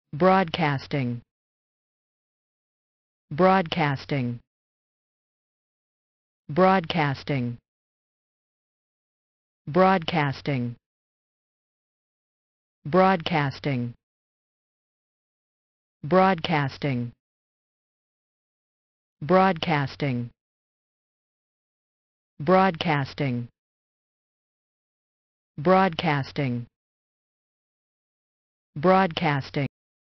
How to Pronounce "broadcasting"
Learn the proper pronunciation of "broadcasting"